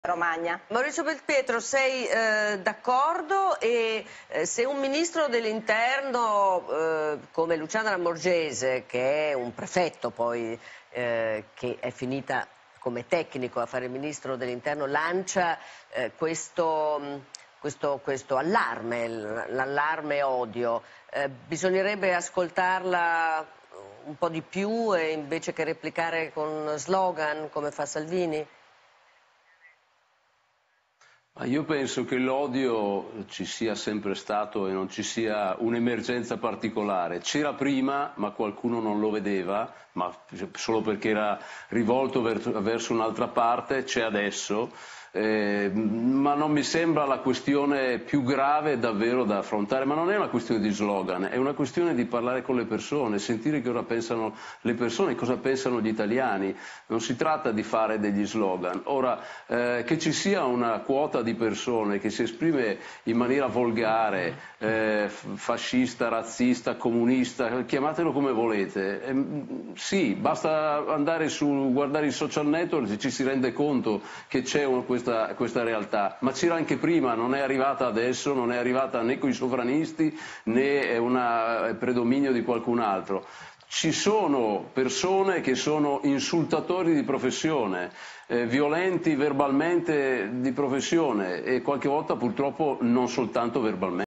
Maurizio Belpietro: "Sono d'accordo con Salvini, l'odio non mi sembra la questione più grave ...
Maurizio Belpietro (Direttore de La Verità): "L'odio c'è sempre stato e non c'è un'emergenza particolare, non mi sembra la questione più grave da affrontare, non si tratta di slogan".